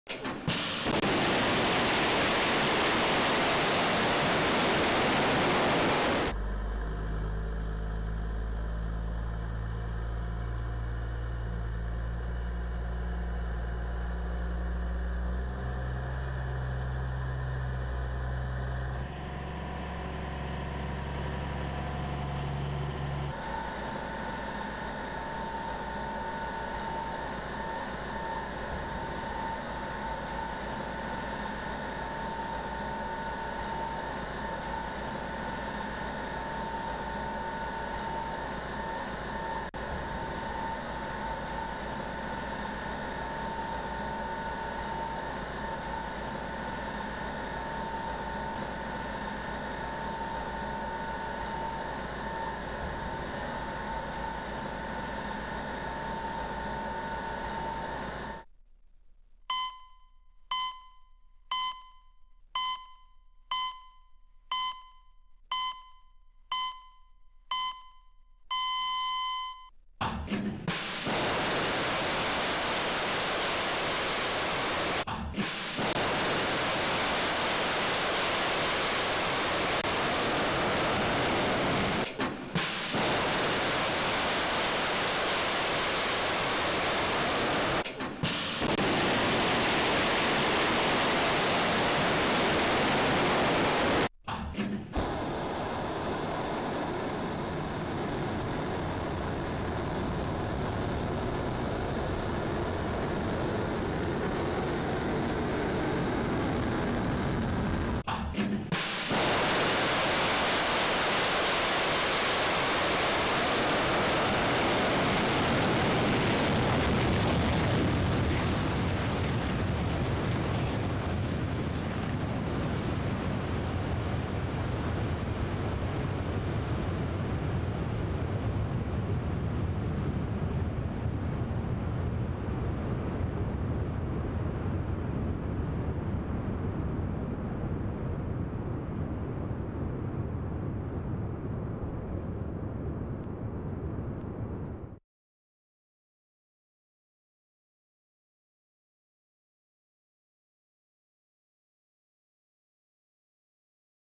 北朝鮮がＩＣＢＭ再発射.
朝鮮中央テレビは２９日午後、火星１４の発射の瞬間をとらえた映像を放映。ミサイルがオレンジ色の炎を放ちながら暗闇を突き抜けていく映像を繰り返し流した。
発射実験は２８日夜、金正恩朝鮮労働党委員長立ち会いの下で行われ、「成功した」と伝えられた。【朝鮮中央テレビより】